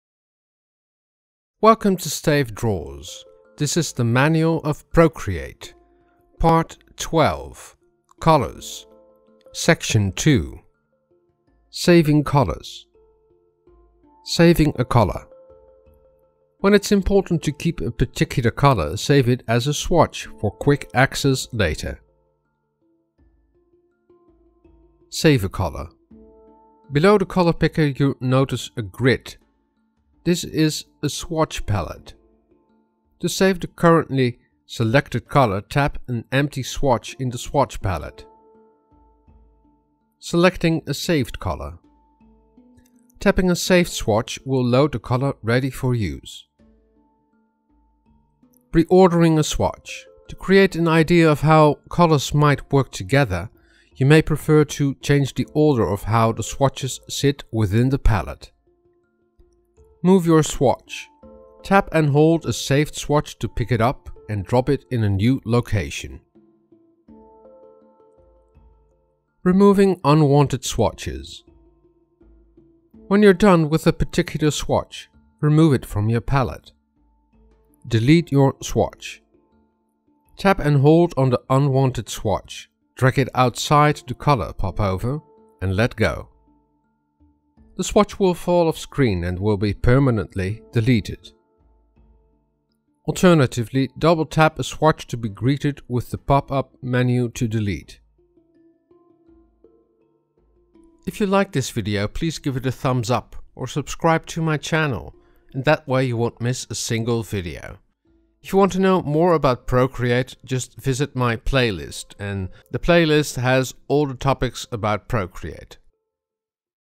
Procreate Tutorial Part 12: Color - Saving Colors
Procreate Tutorial Part 12: Color Section 2 Saving Colors
How to save colours in the iPad drawing app Procreate.

In this video of the Procreate Manual Playlist I show how you can save your colors as swatches.